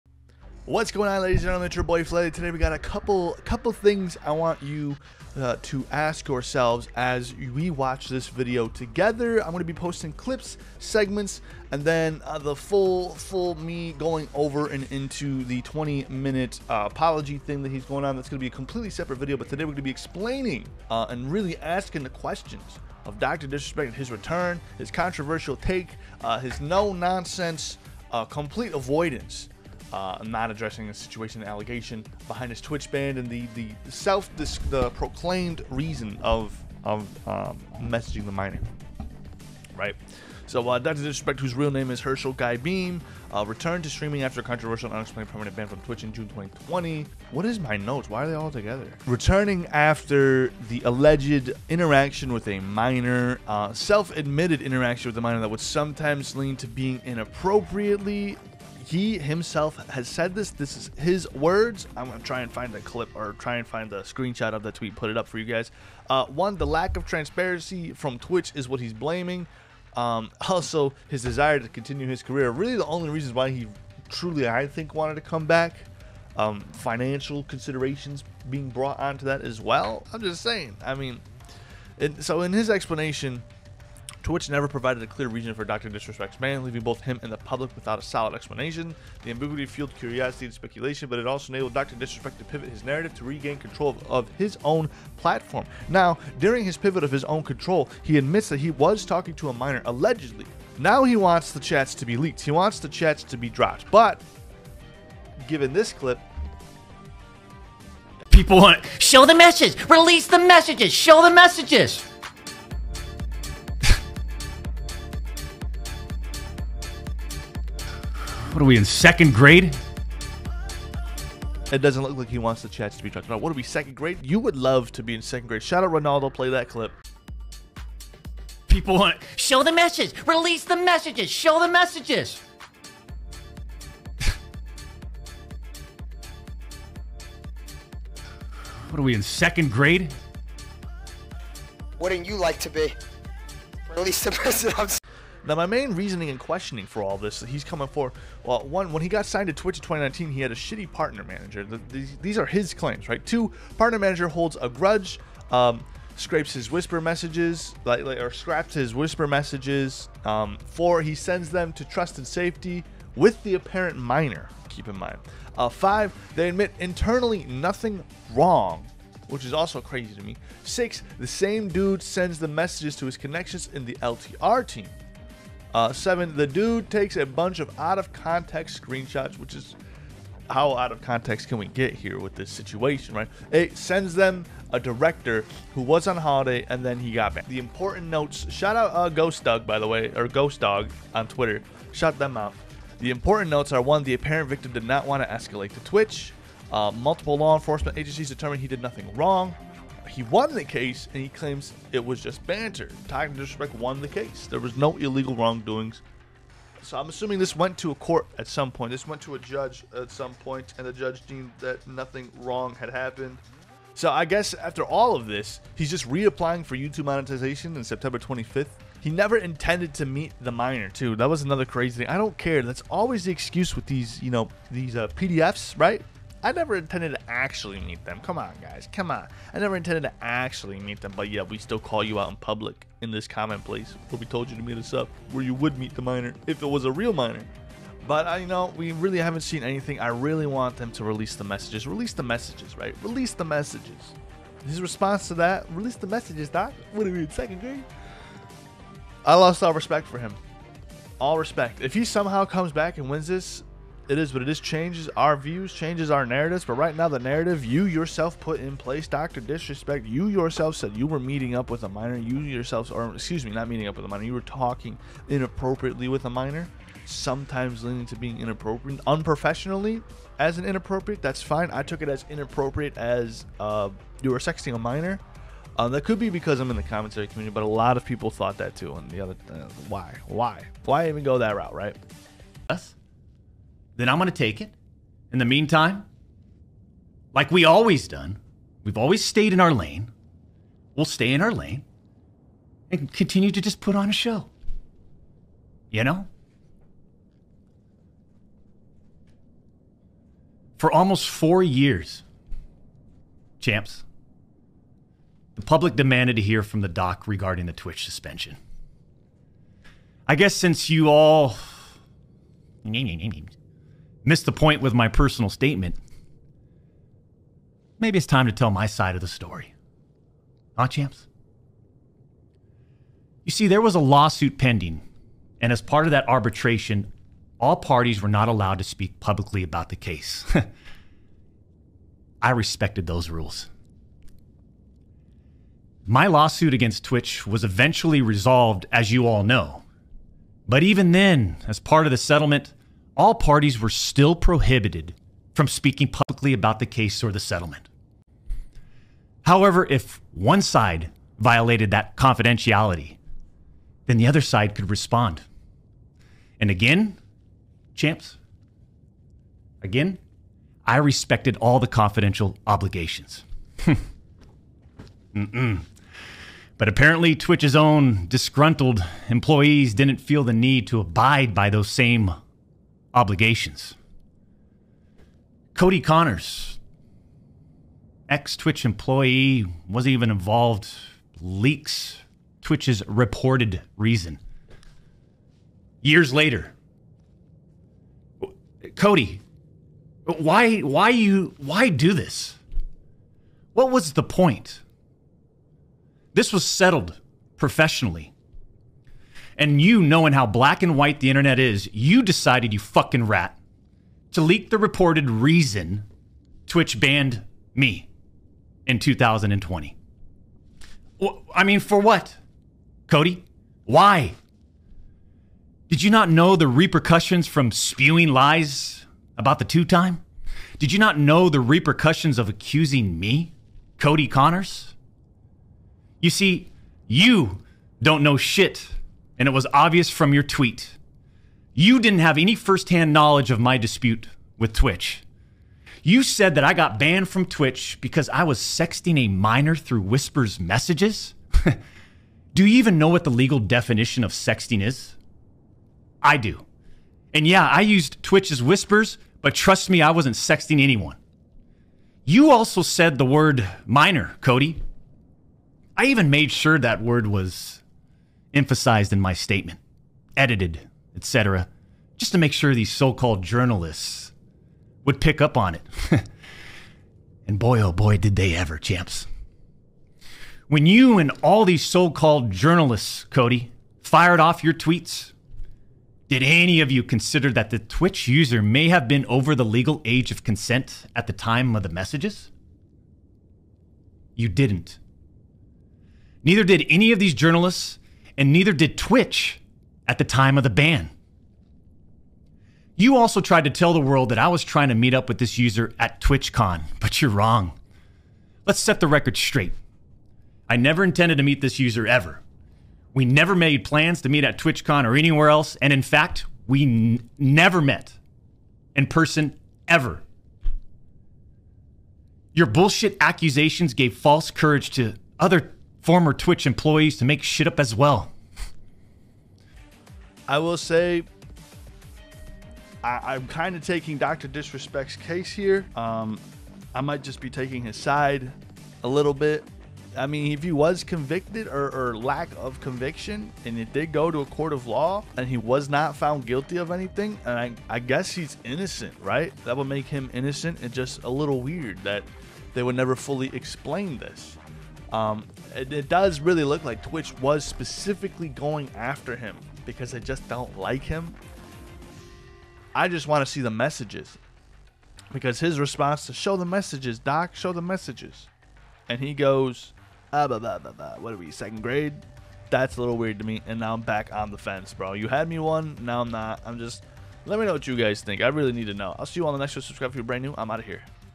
The Doc is Back: Dr Disrespect’s Shocking Return After Allegations
In this video, we dive deep into Dr Disrespect's dramatic return to streaming following the explosive allegations and his mysterious Twitch ban. After months of silence, the 'Two-Time' is back and stronger than ever, but questions still remain. did he overcome the controversy? , and what does this mean for his career going forward? Join us as we break down the timeline, address the rumors, and explore what the future holds for one of gaming’s most iconic personalities!